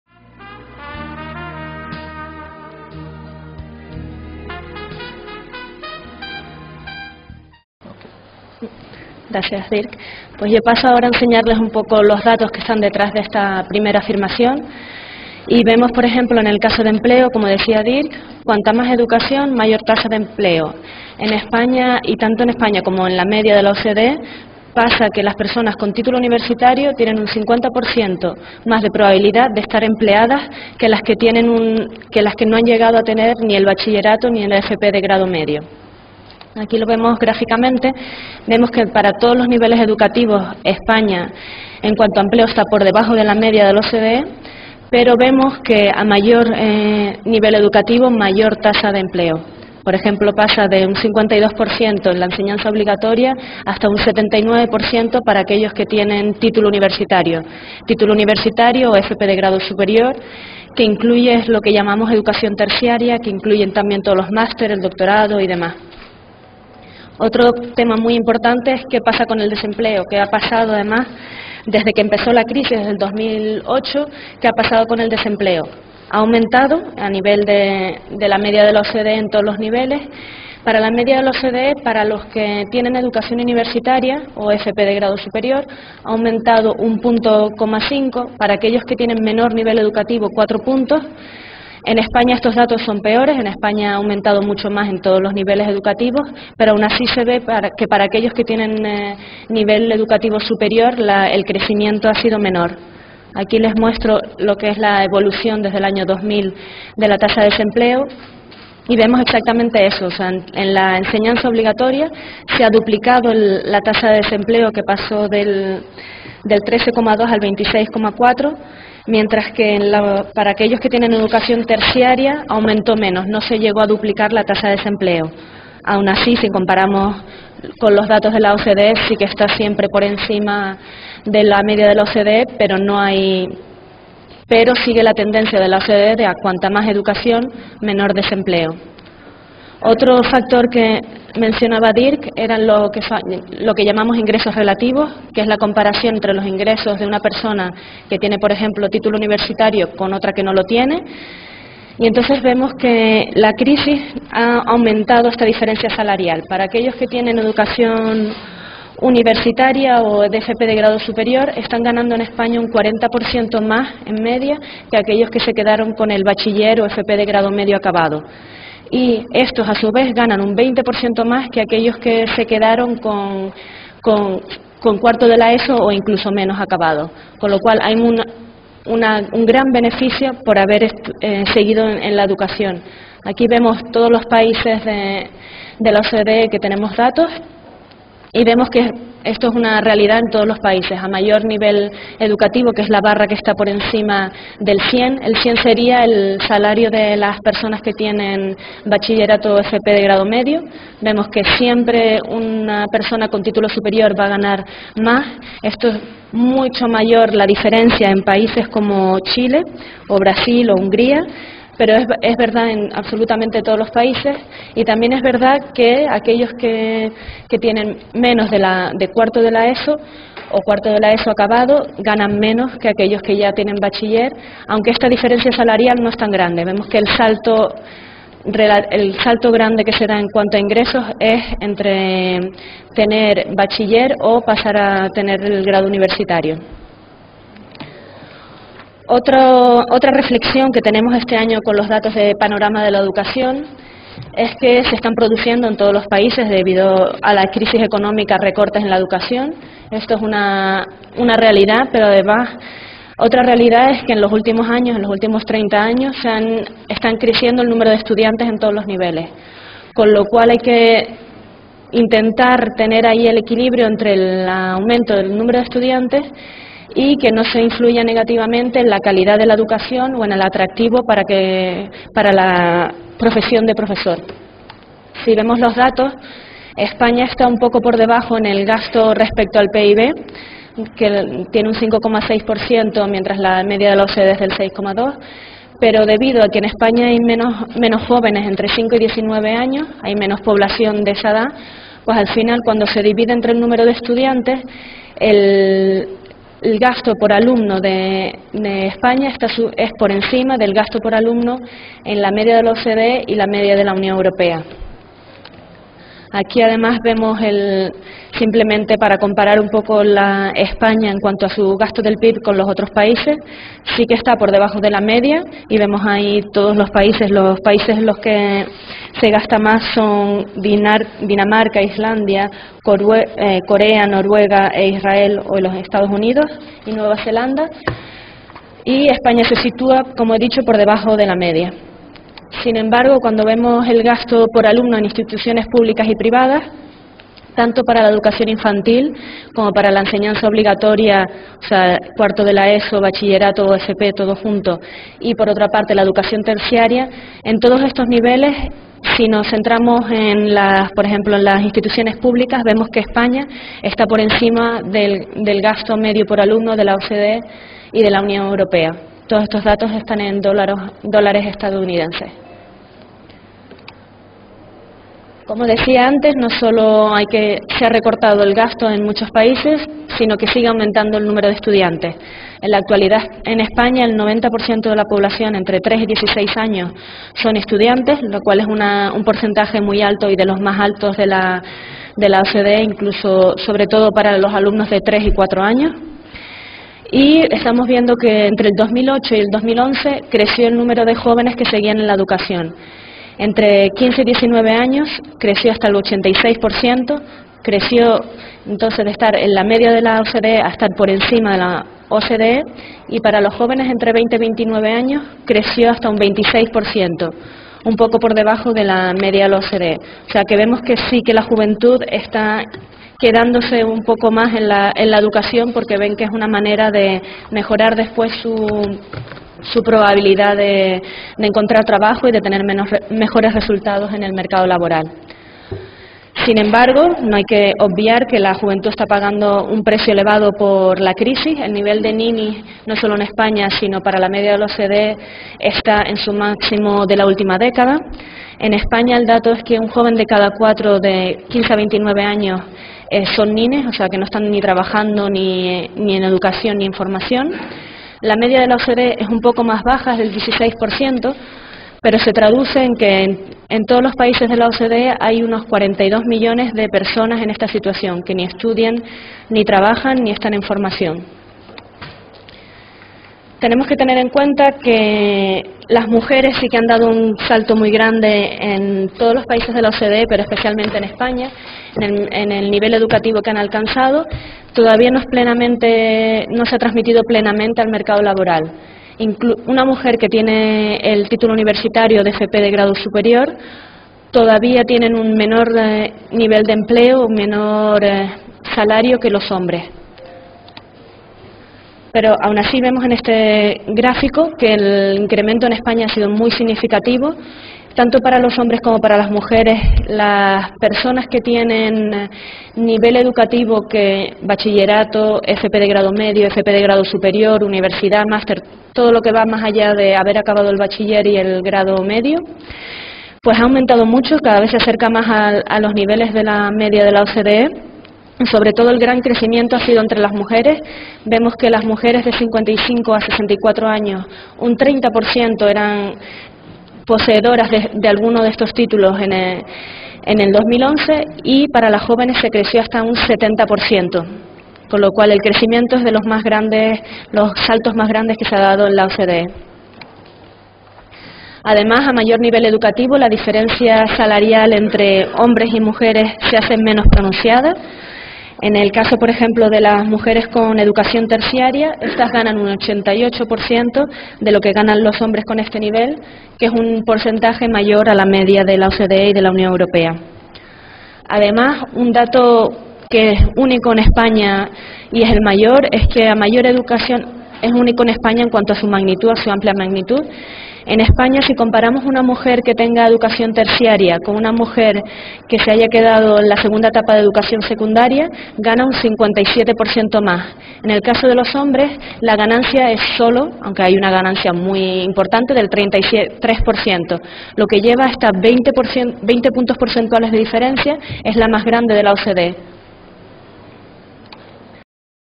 INEE MECD. Indicadores OCDE (2):Situación de la educación española
El martes 25 de junio, se presentaron en Madrid las estadísticas anuales de la OCDE. En este acto, se radiografió la actualidad educativa en España atendiendo a indicadores como el nivel de inversión educativa como porcentaje del PIB, el salario de los profesores, el nivel de estudios de la población adulta, o el porcentaje de estudiantes que continuan en el sistema educativo tras la educación obligatoria.